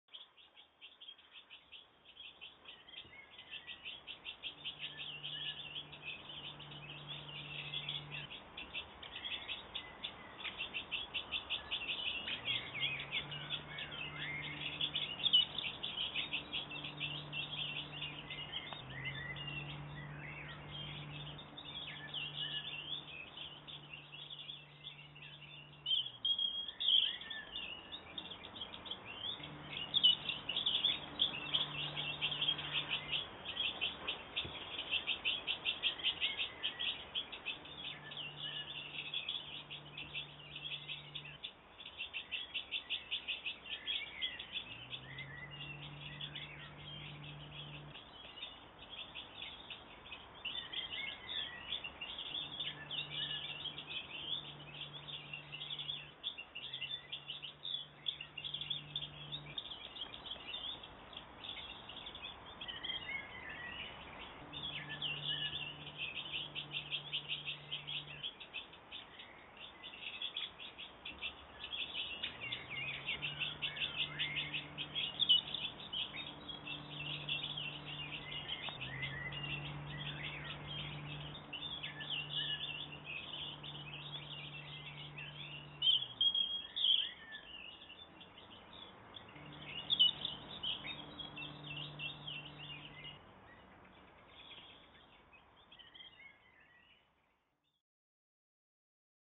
EUROPEAN GOLDFINCHES -- Male & Female - How to tell then apart
Male & Female Goldfinches, their distinguishing features.